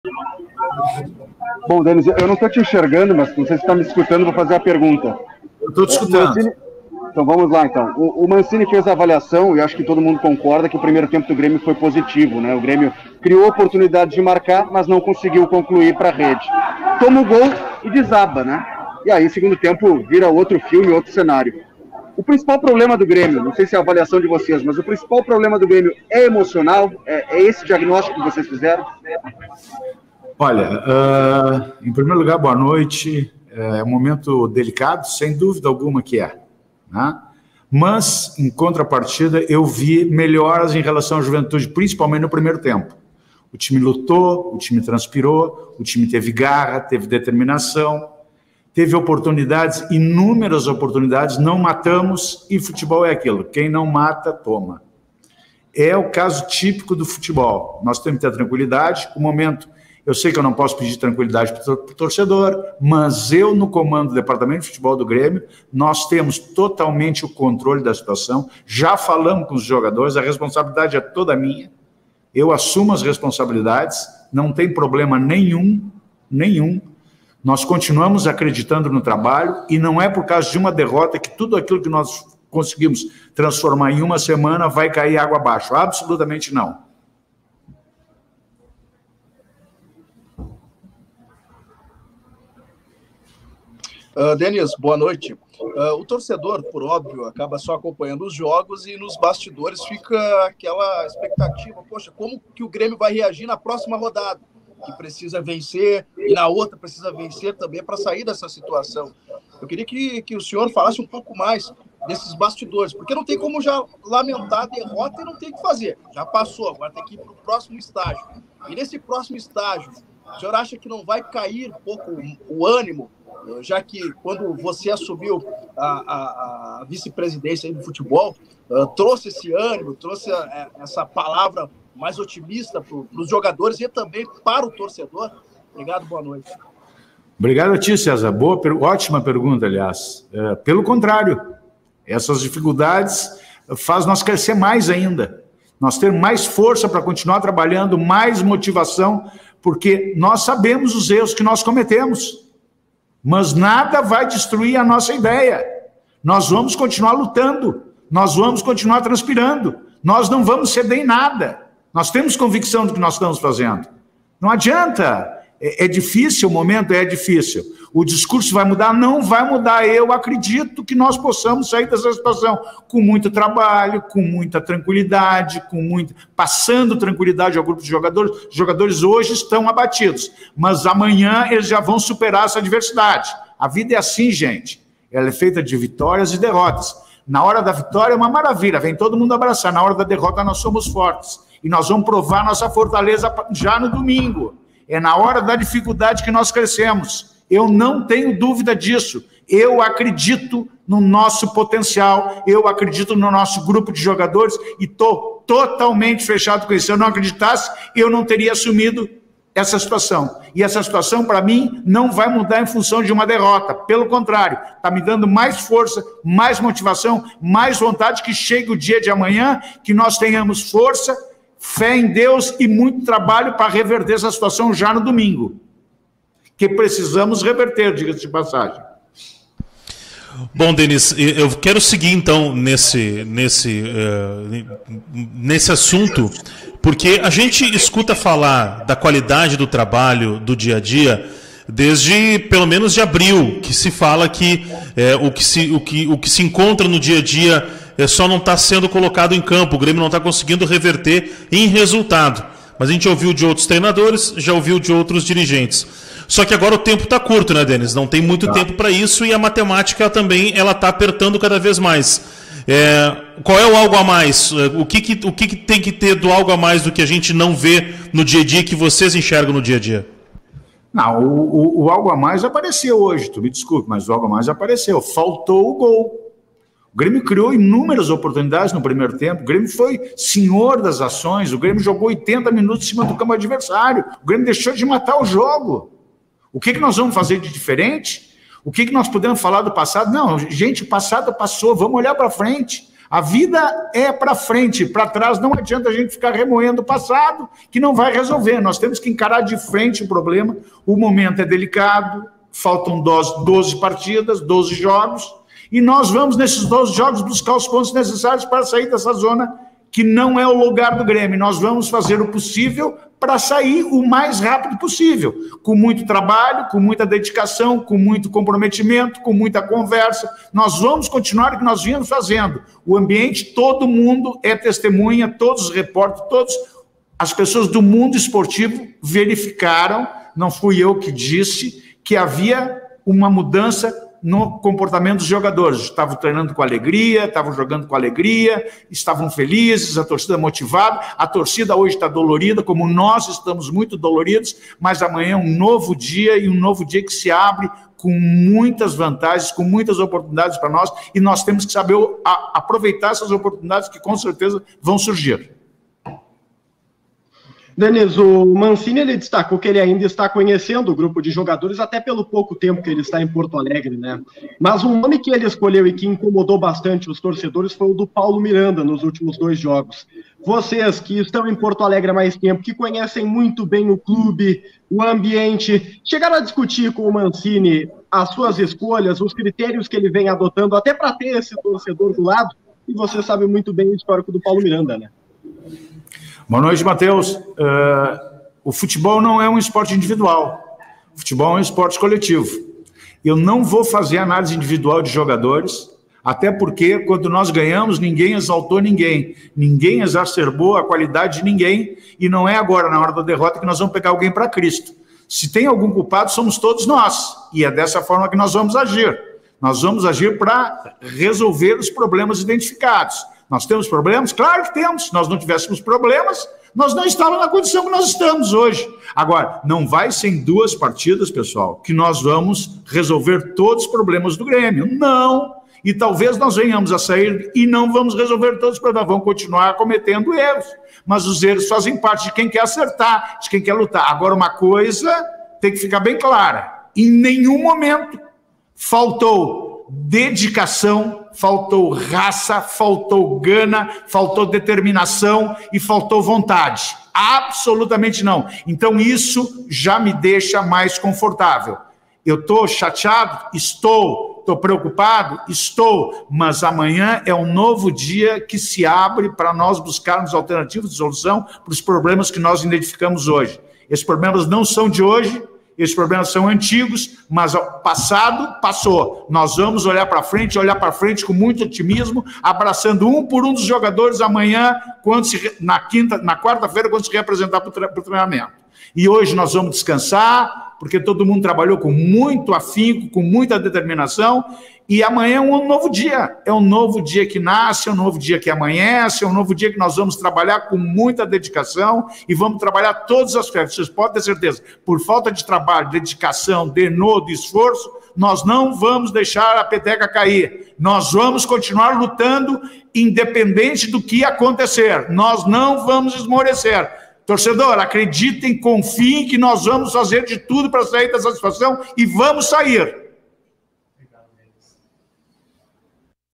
DÊNIS ABRAHÃO FALA APÓS DERROTA DO GRÊMIO PARA O ATLÉTICO-GO: "ME DEU MAIS MOTIVAÇÃO!!!"
Imagens: Grêmio TV